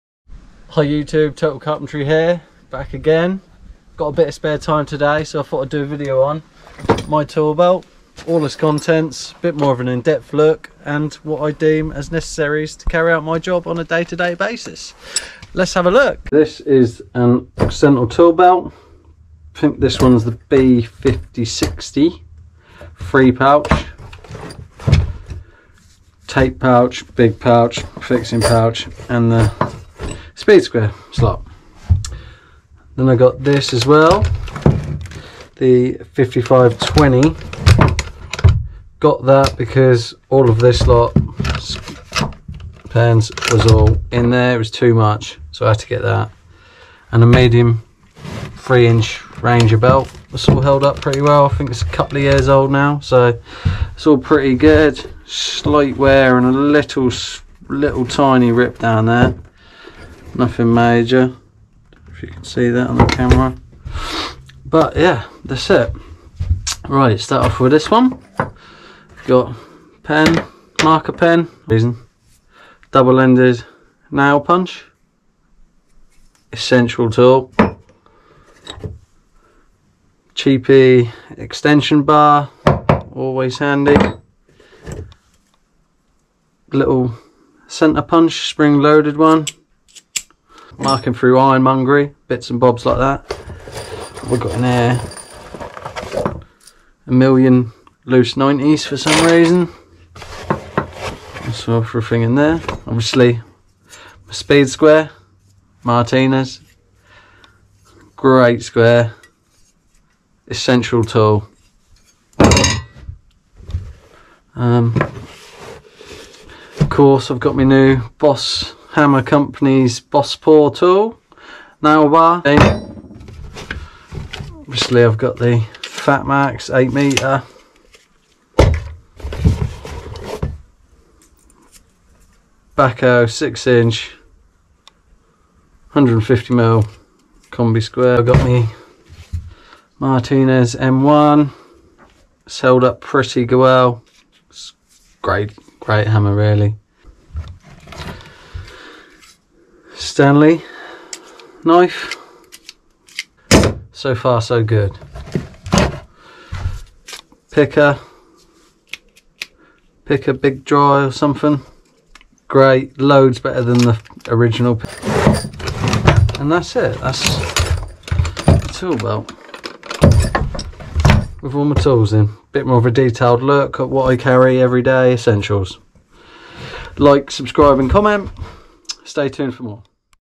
Carpenters Occidental leather tool belt and contents
A quick video on what I carry in my tool belt on a daily basis,